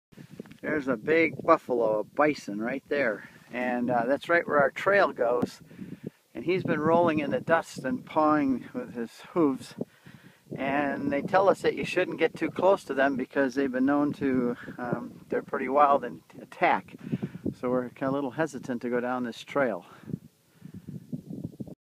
video4.mov: 2012-10-16 Petrified Forest & Buffalo
We are here in North Dakota giving talks at a church in Beach, North Dakota. As long as we are staying right on the edge of the Theodore Roosevelt State Park, we decided to hike back into the Badlands and the grasslands of North Dakota and see the Petrified Forest.
Along the way we came upon several bull bison and were somewhat concerned because the Park rangers say that they can be very dangerous. So we walked slowly. We also had an escape plan in case they charged.
 It was a marvelous hike walking in landscape like we've never seen before. At times it almost seemed like we are on another planet.